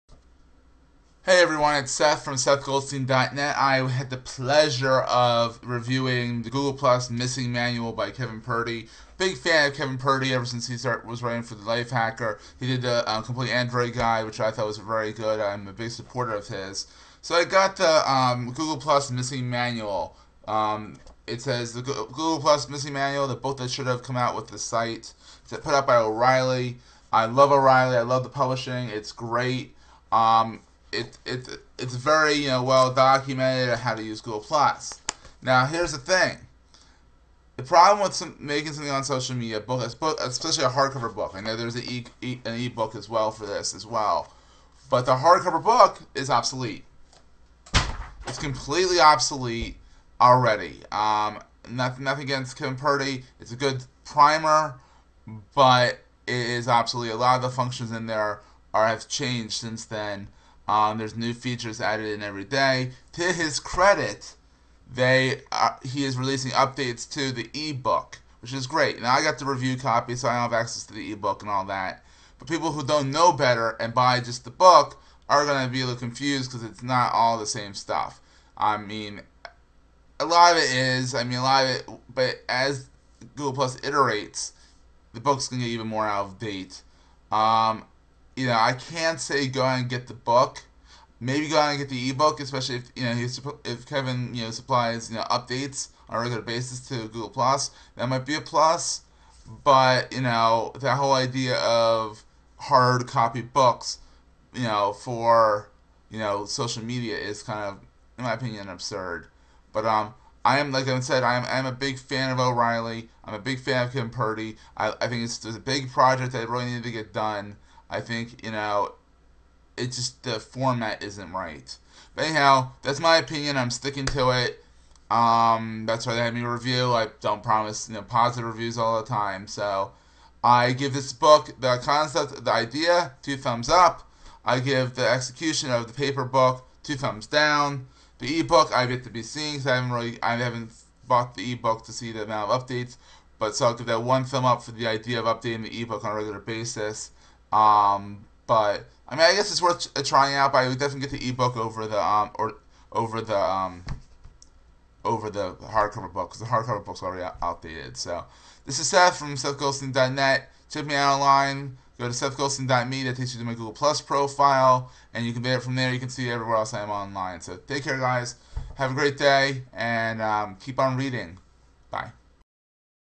Book Review - The Google Plus Missing Manual by Kevin Purdy
O'Reilly sent me a copy of Kevin Purdy's new book "Google+ The Missing Manual." This is my review.